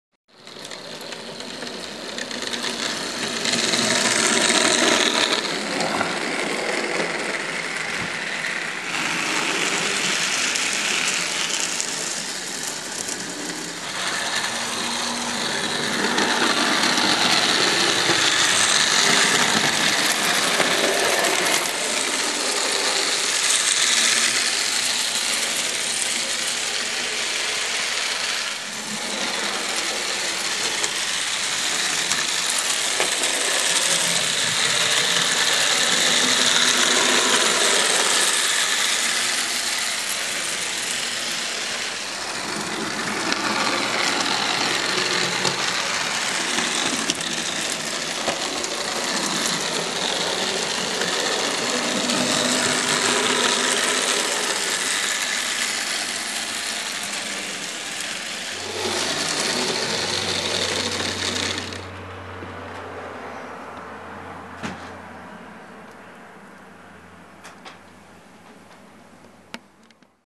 Papp -Bahn Draisine Gn15 zweiter Versuch
Probefahrt der zweiten Version.
Rohbau aus Plakatkarton
mit FLEISCHMANN Fahrwerk 1306